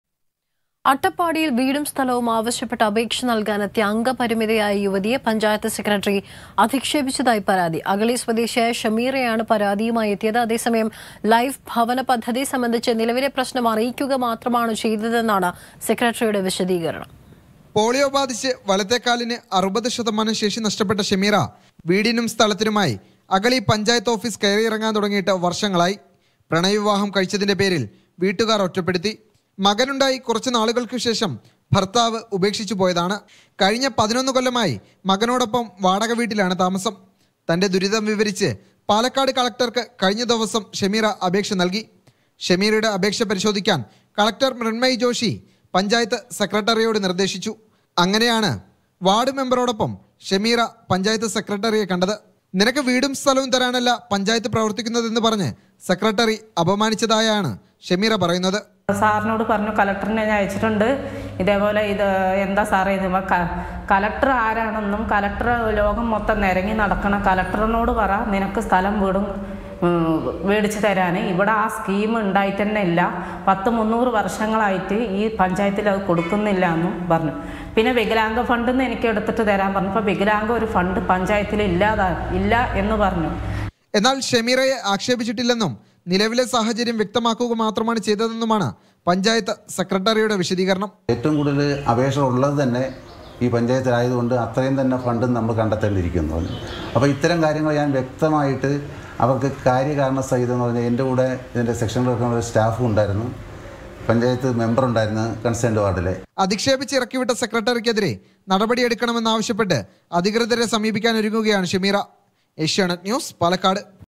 'വീടും സ്ഥലവും ആവശ്യപ്പെട്ട് പഞ്ചായത്ത് ഓഫീസിലെത്തിയപ്പോള്‍ അധിക്ഷേപിച്ചു'
വീടും സ്ഥലവും ആവശ്യപ്പെട്ട് പഞ്ചായത്ത് ഓഫീസിലെത്തിയപ്പോള്‍ അധിക്ഷേപിച്ചു; പരാതിയുമായി അംഗപരിമിത
 Complaint against panchayat Secretary